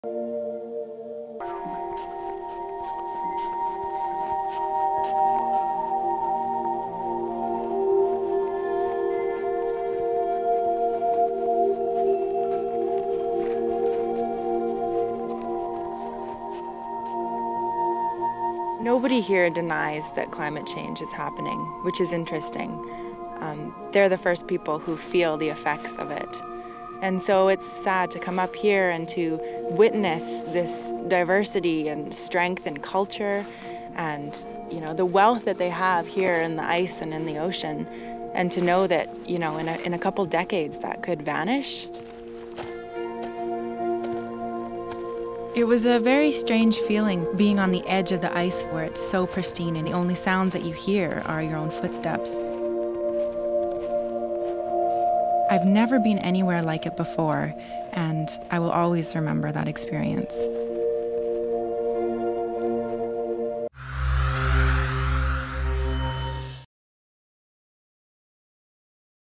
The Arctic and Climate Change
Sarika Suzuki visit Canada's arctic.